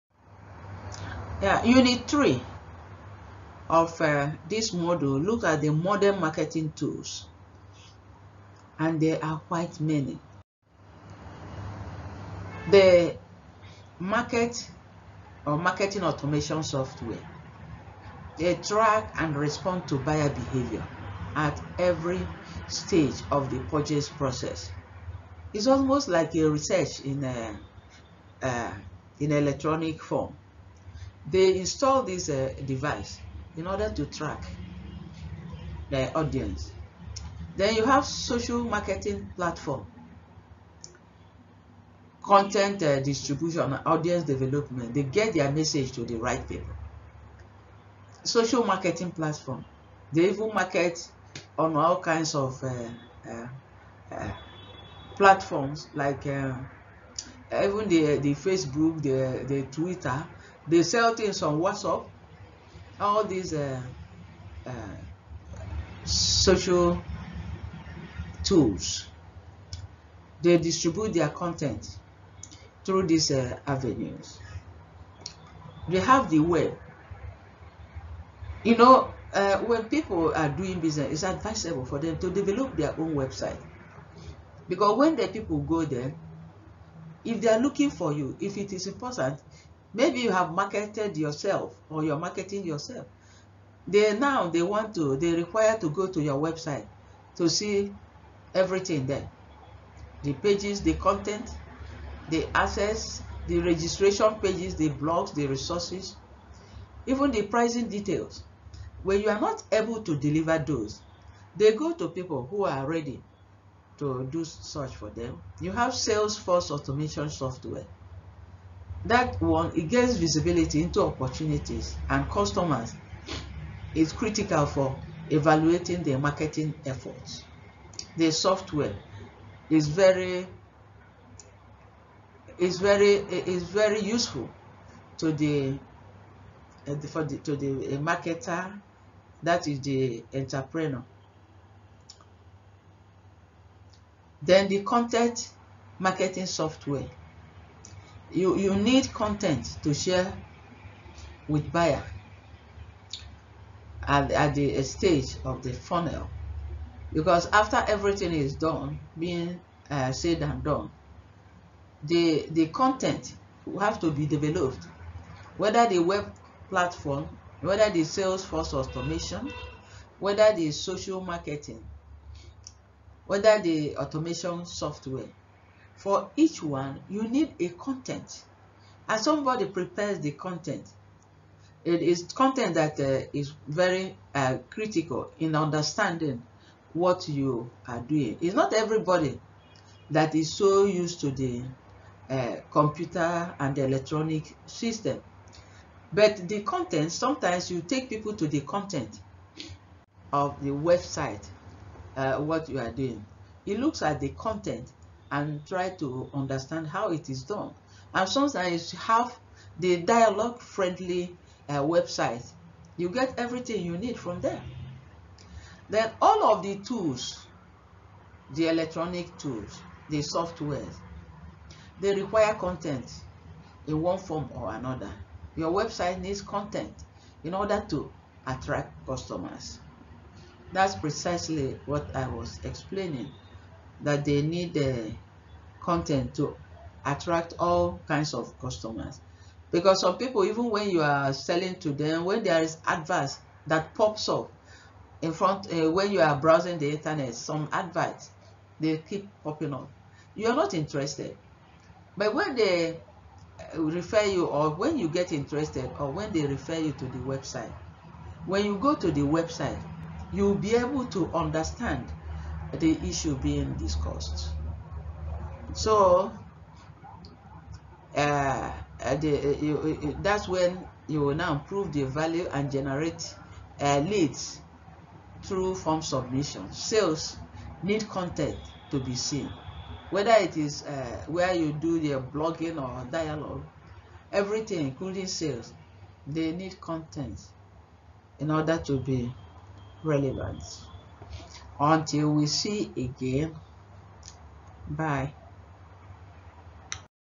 Module 4 Unit 3: Modern Marketing Tools
Modern Marketing is a holistic, adaptive methodology that connects brands with real customers and drives business results by blending strategy, creative, technology, and analysis